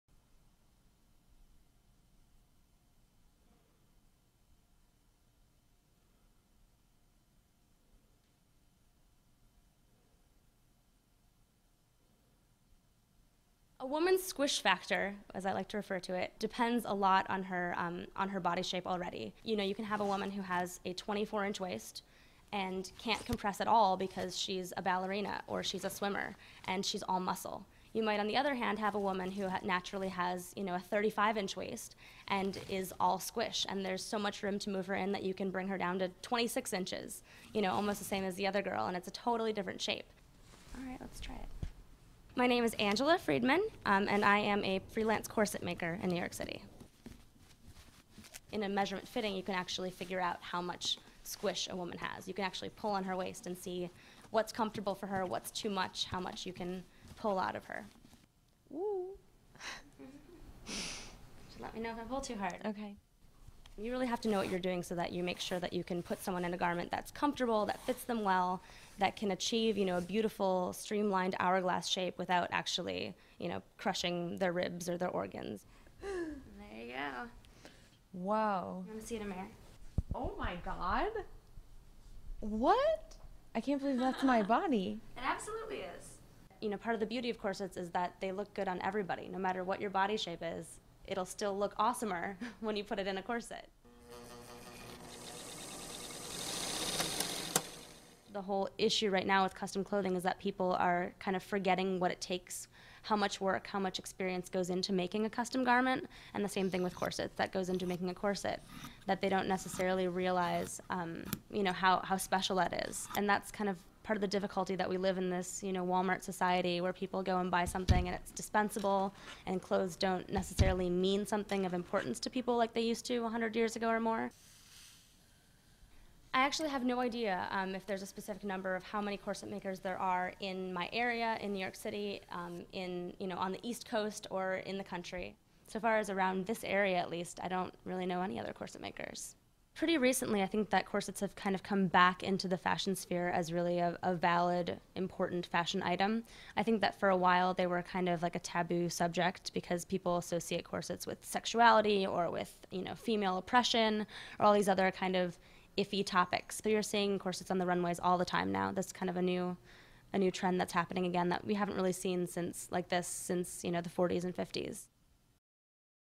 The Corset Maker
Friedman received her degree in costume technology from The University of Northern Carolina and currently manages the women's costume department at the New York City Ballet. The price of her custom corsets start at $300.